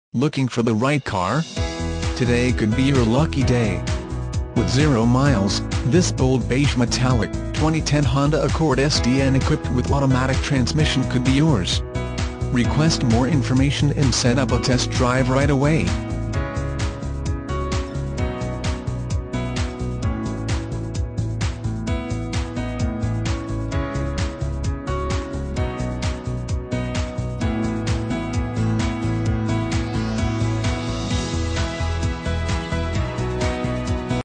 2010 HONDA ACCORD SDN Littleton, CO
2010 HONDA ACCORD SDN Littleton, CO
Stock #01979

You'll love this 2010 Honda Accord Sdn. This is a car you'll want to take home. With 6 miles, it features automatic transmission and an exterior color of Bold Beige Metallic. Call us and be the first to open the car door today!